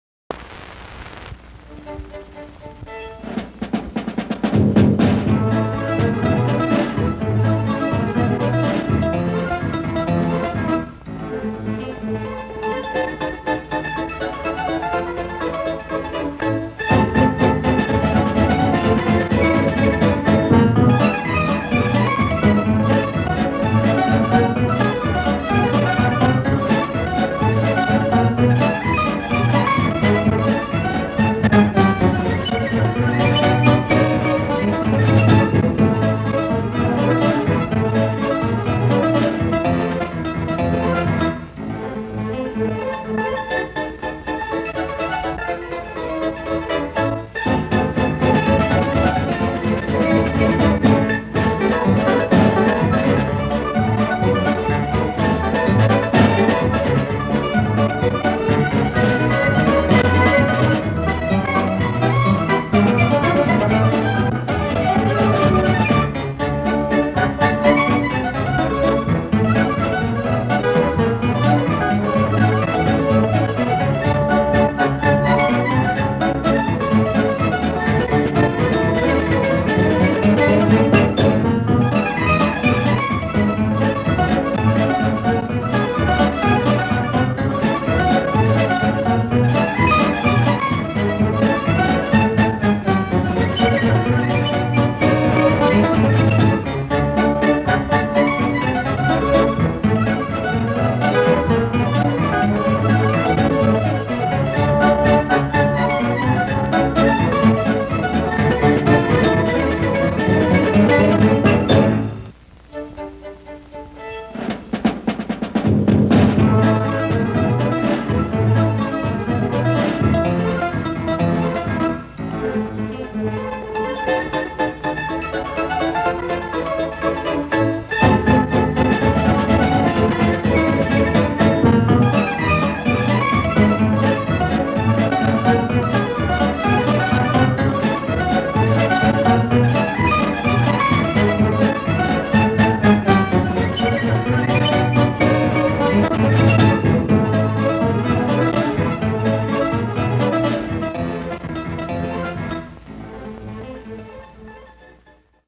Carless - a RES film
A Stop frame animation film I made. Filmed on super 8.